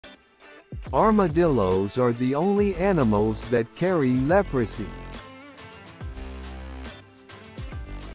❓ ArmadilloTrivia: Did you know armadillos are linked to leprosy transmission? 🤔🦏
🦏🤝 Armadillos play a unique role in leprosy transmission! They are the only known non-human animal that can carry the bacteria responsible for leprosy, making them a significant factor in the disease's spread. 🦾🦠

🌟 Share this intriguing fact with your friends and let them discover the unexpected connection between armadillos and leprosy! 🎬🔔

Save